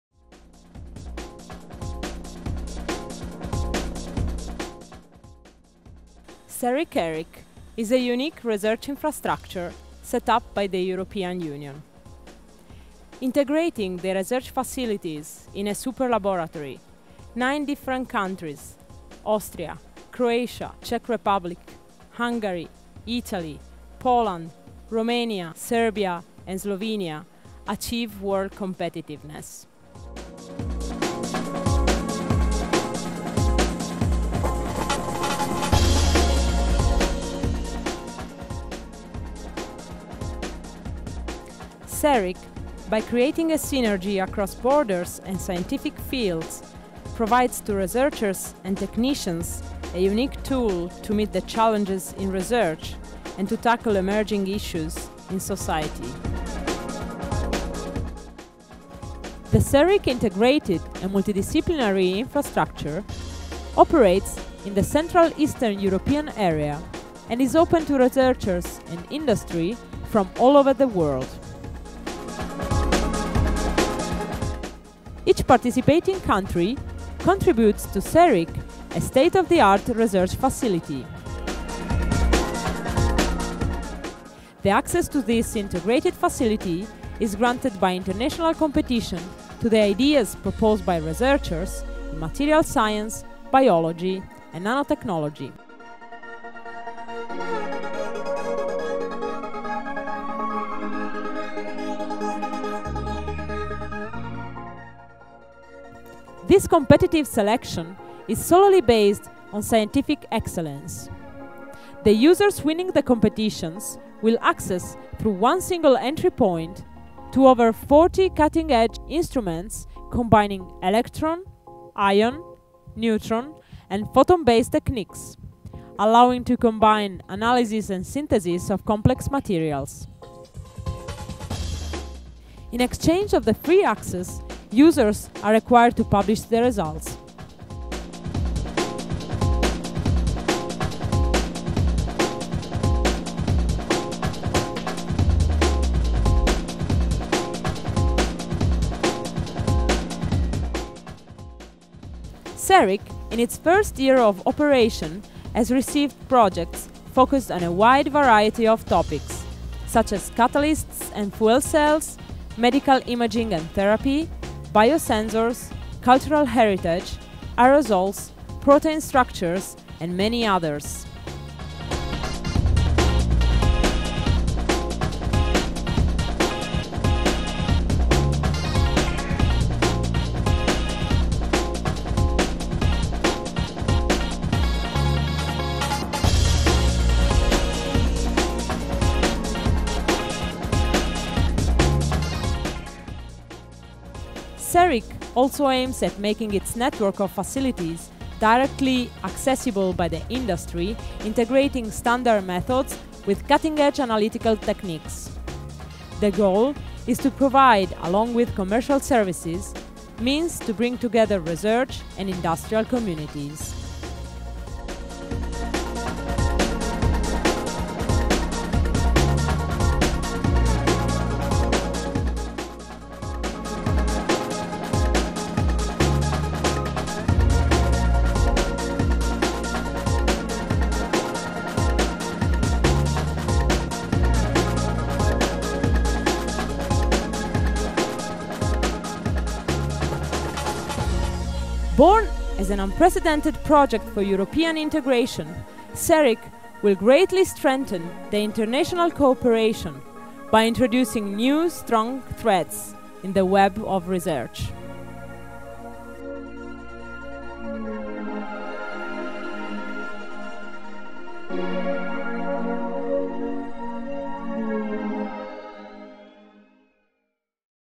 CERIC-ERIC, the Central European Research Infrastructure Consortium
Discover CERIC-ERIC and its research facilities open in Central and Eastern Europe for research in all fields of materials and biomaterials science, and nanotechnology.

VIDEO CREDITS:
*recordings: CERIC and partners (Graz University of Technology, Ruđer Bošković Institut, Charles University Prague, Budapest Neutron Centre - Centre for Energy Research, Elettra Sincrotrone Trieste, National Synchrotron Radiation SOLARIS, National Institute of Materials Physics, Slovenian NMR Centre - National Institute of Chemistry)
*filming at CERIC headquarters: Onur Tevfik Mentes (Elettra Crowd Team)
*speaker, (post-)production / video-editing: Nicoletta Carboni, CERIC-ERIC